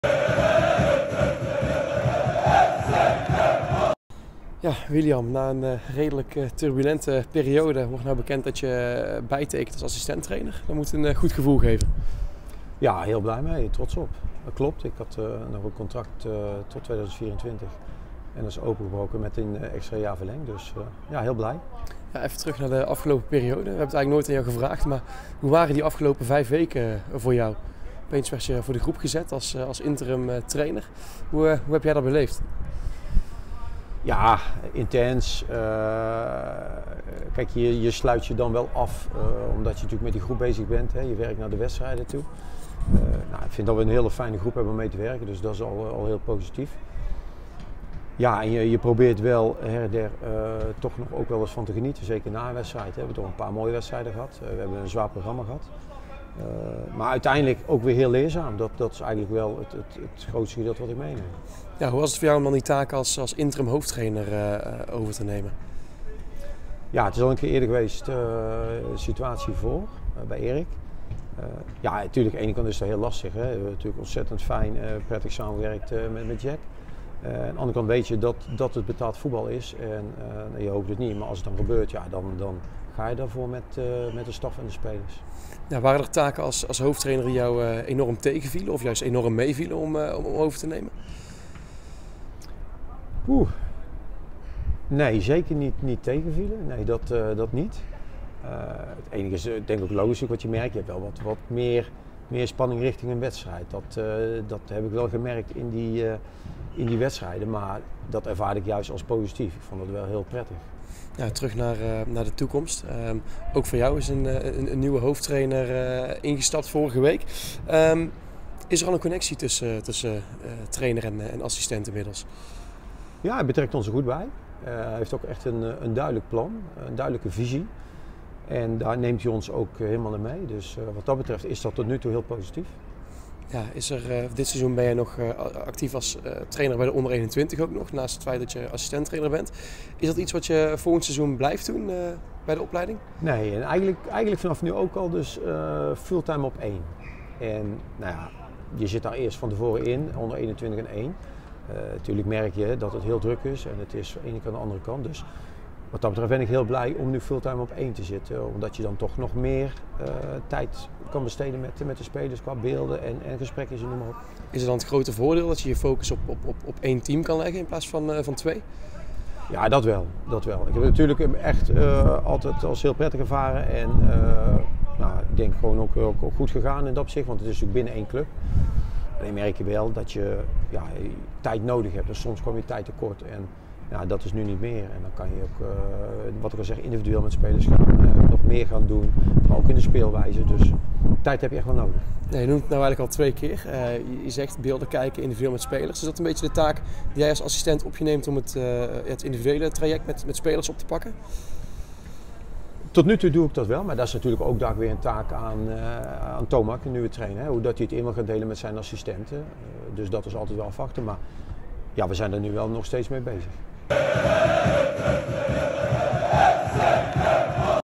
🎙INTERVIEW | William van Overbeek verlengt als assistent bij FC Den Bosch
Interview met assistent-trainer William van Overbeek